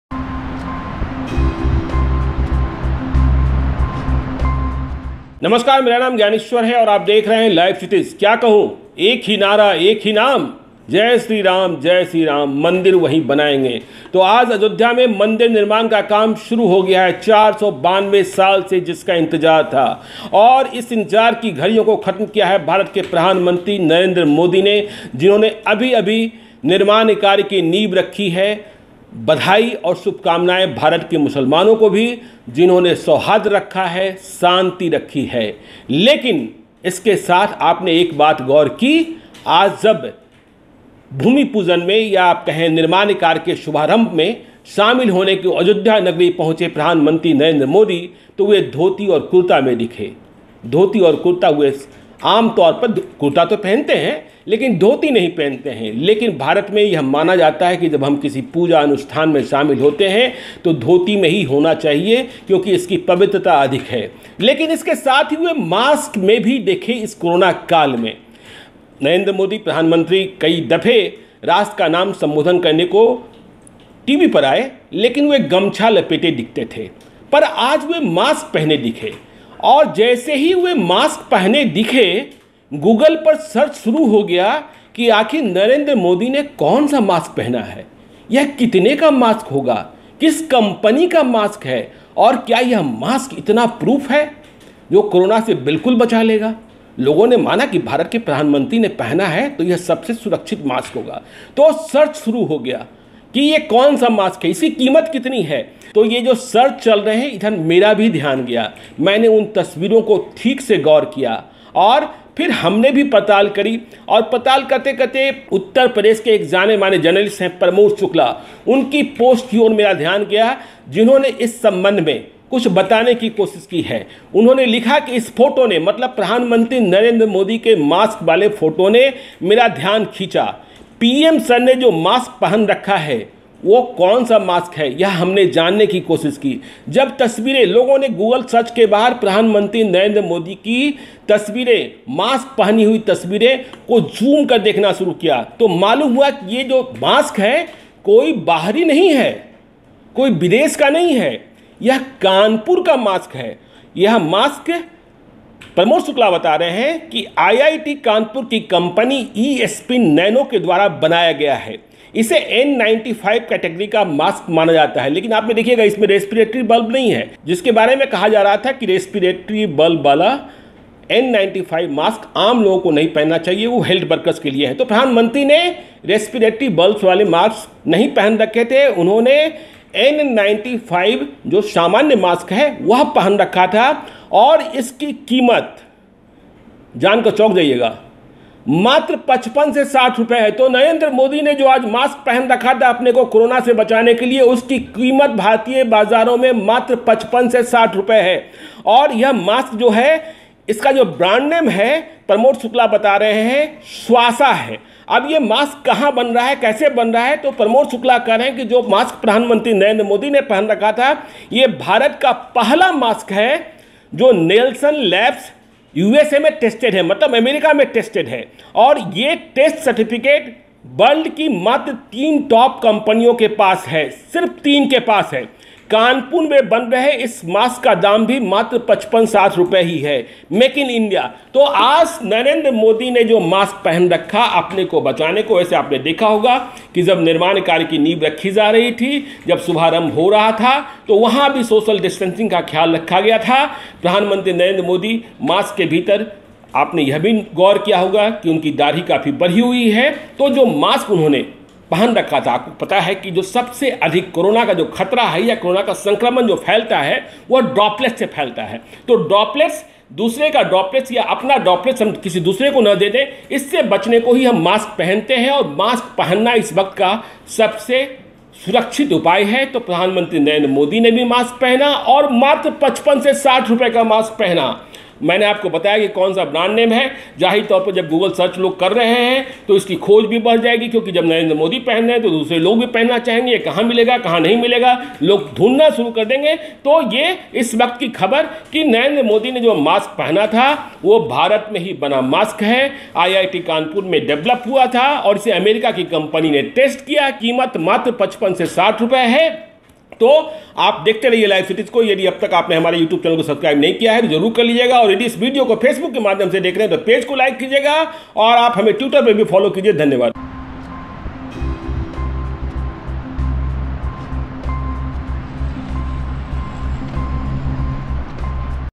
Ayodhya में कोरो ना से बचने को Narendra Modi ने पहन रखा था कौन सा मास्क, चल गया पता, कीमत ये है..
Ayodhya में कोरो ना से बचने को Narendra Modi ने पहन रखा था कौन सा मास्क, चल गया पता, कीमत ये है | Bihar News | LiveCities